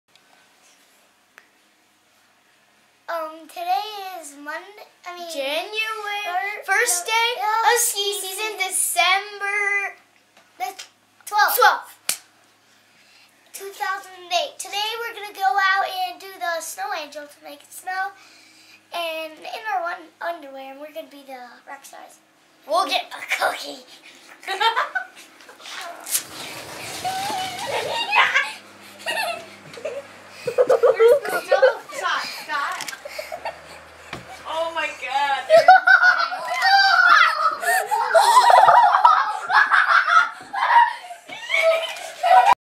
snowangels to make it snow.mpg
Today was the first day of ski team and it was snowing at Alpine Meadows. The kids thought they'd help it keep snowing.